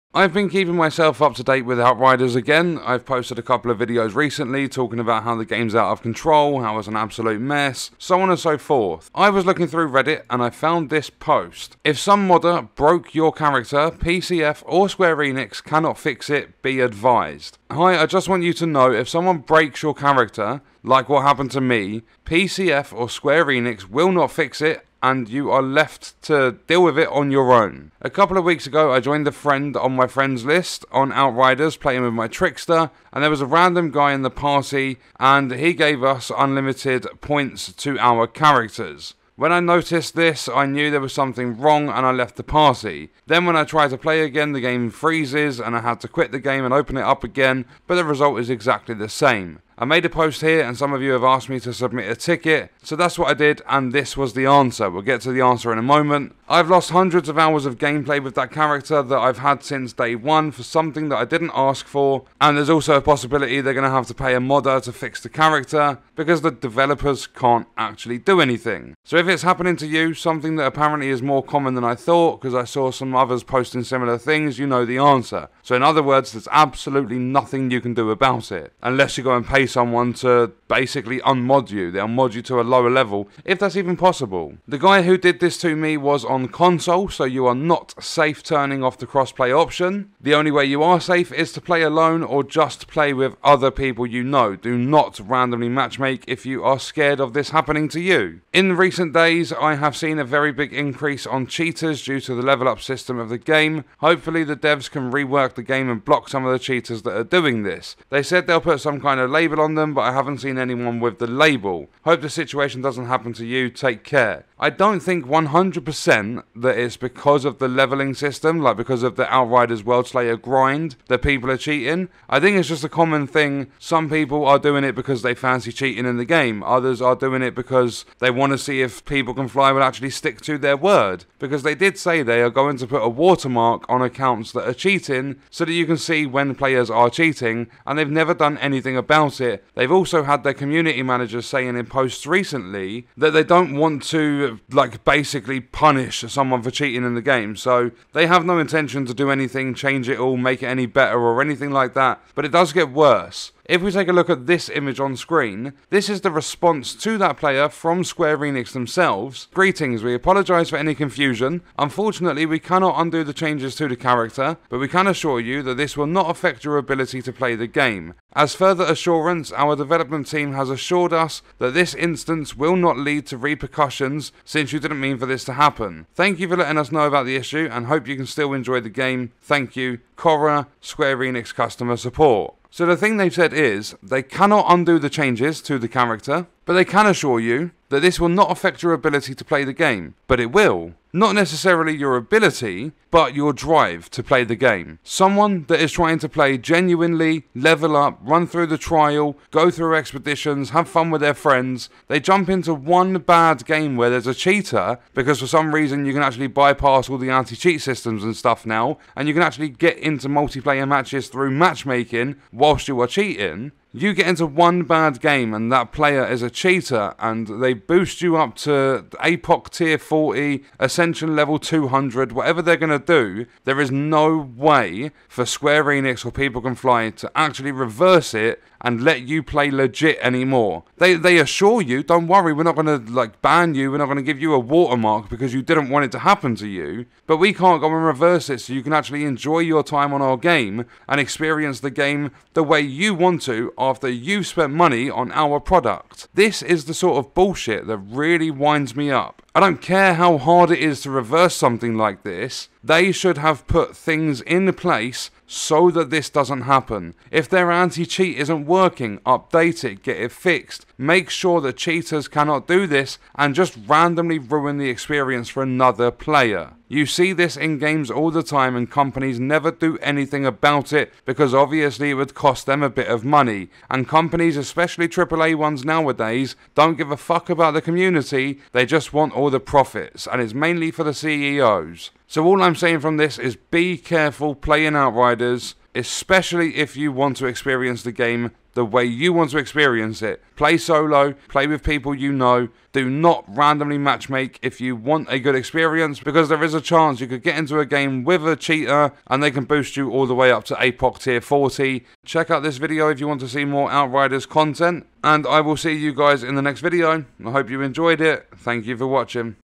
Outriders Worldslayer MUST WATCH! Outriders Cheats are BREAKING the Game
Outriders Worldslayer MUST WATCH! Outriders Cheats are BREAKING the Game. In this video we are going through a Reddit post that highlights a huge problem with Outriders since the release of the Outriders Worldslayer expansion. I understand it takes time to work on patches/updates but when there is barely any notice taken to problems in a game it becomes much more widespread and people start leaving the game. Since the release of the Outriders Worldslayer expansion which wasn't too long ago over half of the concurrent player base from Steam have gone again and there are multiple causes but one of them we are talking about in this video and that is Outriders cheats or cheaters. People are able to bypass the anti cheat system and publicly matchmake in Outriders Worldslayer and they are skipping the entire grind for some players.

When the development and publishing teams (People Can Fly and Square Enix) are contacted they leave a response saying it doesn't have any effect on the player and they won't be punished because they didn't want it to happen but that does nothing to help the player that doesn't want to be boosted to max level. This video in my opinion is a must watch because I'm able to warn you about randomly matchmaking if you don't want your experience to be ruined. Outriders cheats are breaking the game and nobody can do anything about it besides playing solo or only with people you know. This just further backs my points in my Outriders Worldslayer review and I don't know whether you would call it cheats or Outriders Worldslayer hacks. Let me know your thoughts about this Outriders Worldslayer cheating problem in the comments below :D